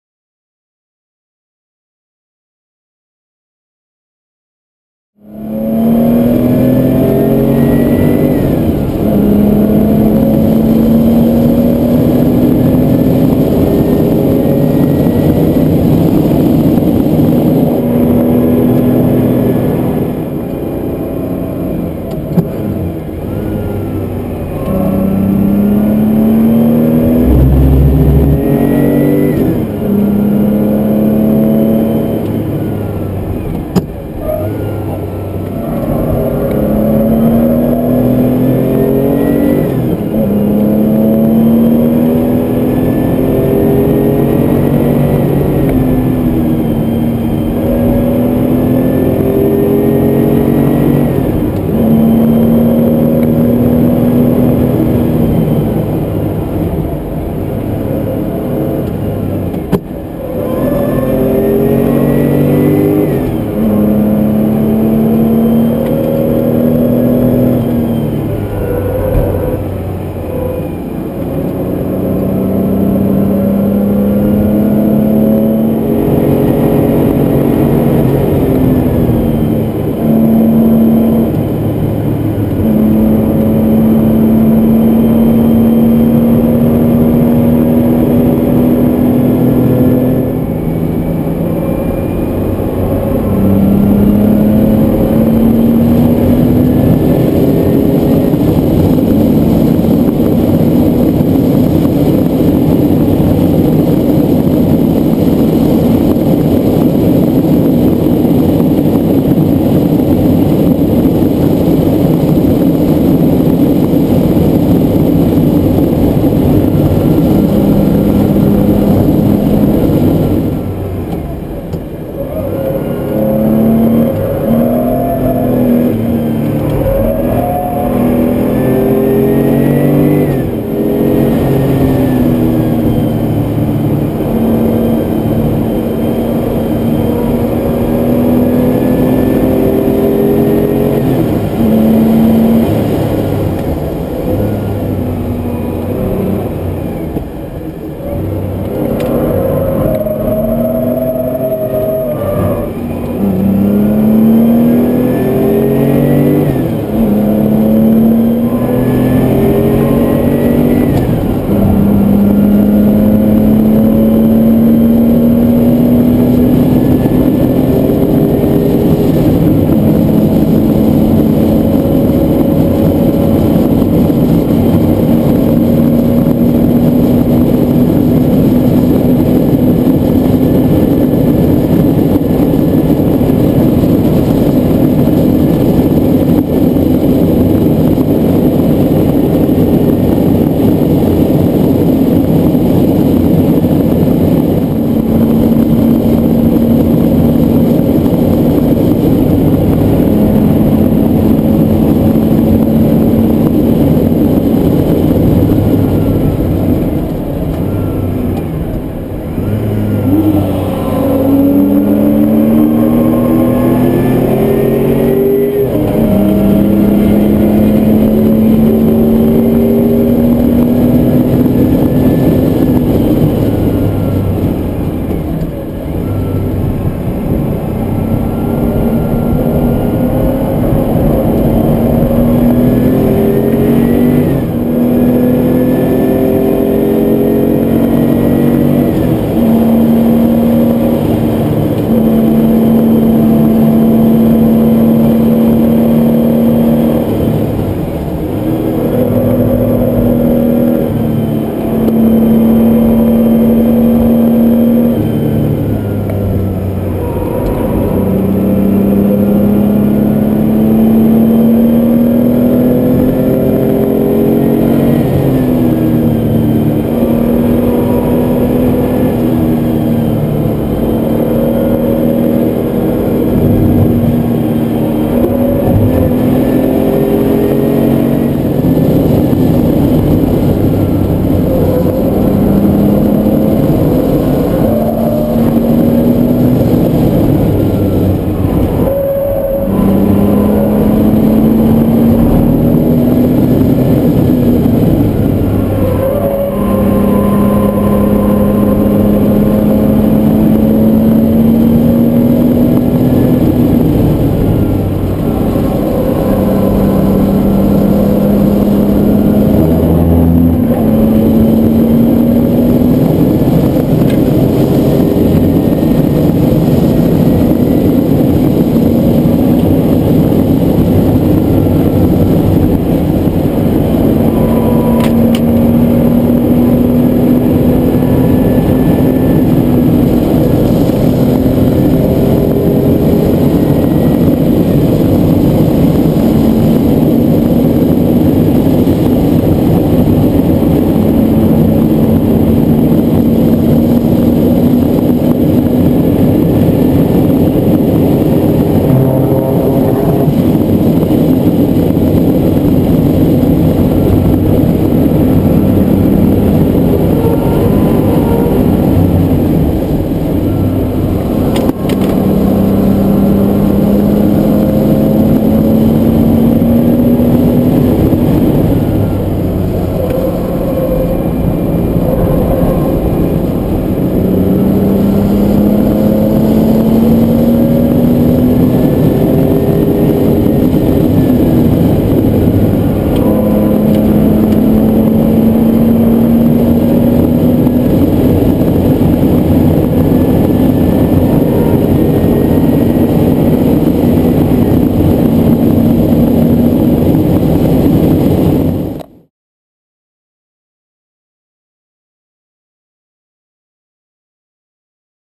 Porsche 911 Carrera RS "Dolly" Nürburgring Nordschleife
Porsche 911 Carrera RS "Dolly" Recreation Nürburgring Nordschleife Laps
Check out @racetracker.de for amazing photos
Check out @mattaboutcars for more car stuff